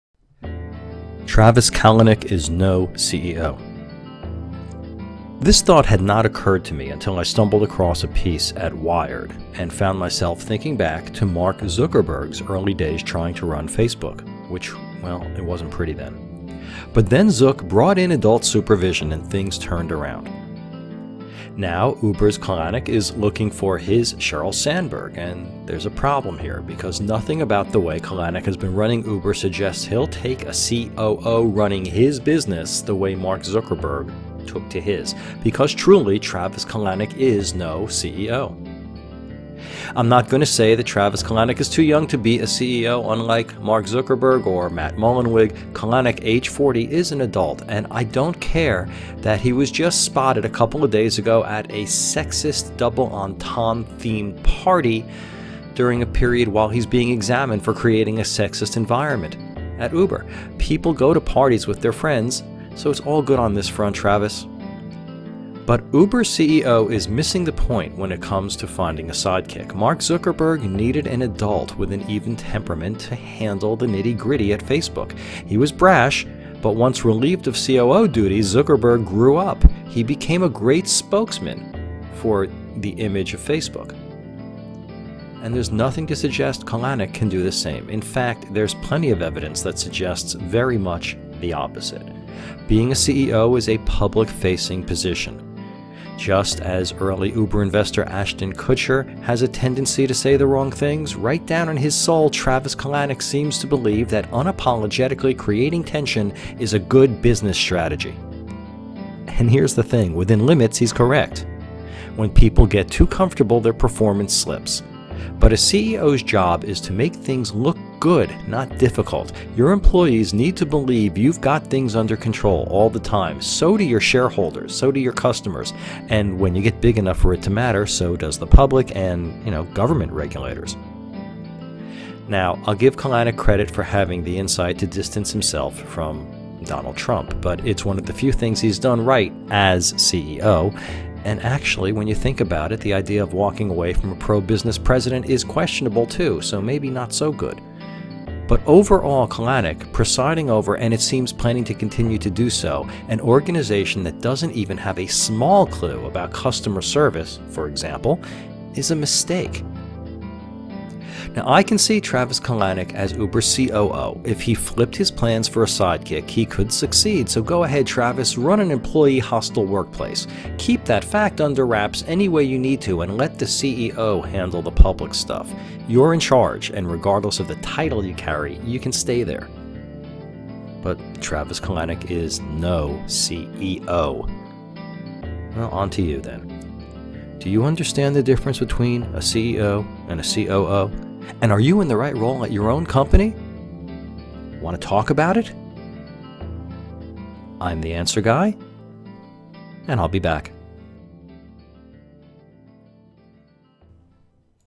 Travis Kalanick is no CEO
Travis Kalanick is no CEO. And Uber's leader is having his "Travis Kalanick is no CEO" moment, even as he searches for a COO to help him out.